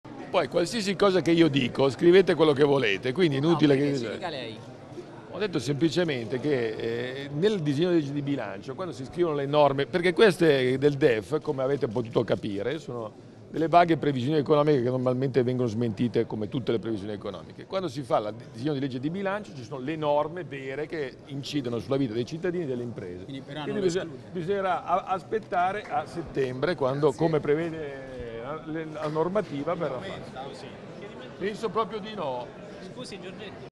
Giorgetti: "Nel Def solo vaghe previsioni, le norme vere solo con la Legge di Bilancio"
(Agenzia Vista) - Roma, 11 Aprile 2019 - "Nel Def ci sono solo vaghe previsioni, bisogna aspettare Settembre, nel Bilancio ci saranno le norme vere e proprie." Così il sottosegretario con delega allo Sport, Giancarlo Giorgetti a margine della conferenza stampa di presentazione degli Internazionali di Tennis di Roma. La conferenza, per la prima volta, si è svolta a Palazzo Chigi.